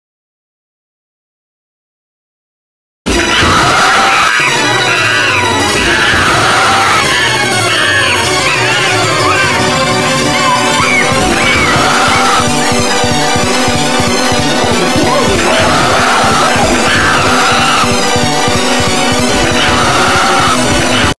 [LOUD WARNING] Fido offers you ‘food’ :D
Song: A mixture of monkey sounds, grandma jumpscare and circus music. :D

Character:

Fido

Happy 2023 :)